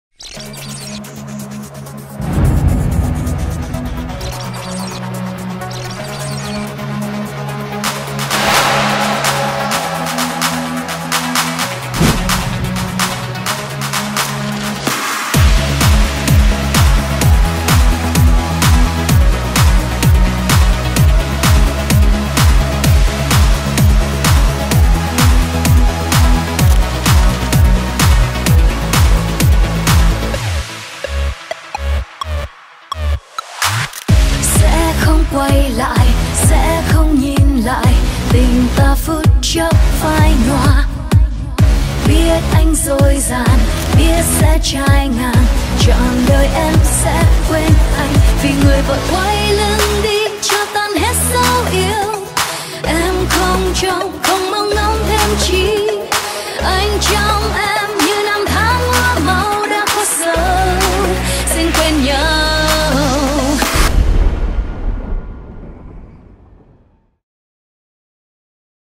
Cuộc thi Khoảnh Khắc Hào Quang - Vòng Chung Kết
4 thí sinh xuất sắc nhất của cuộc thi Khoảnh Khắc Hào Quang đã xuất hiện. Bình chọn cho các thí sinh để chọn ra một gương mặt đại diện các bạn nhé!.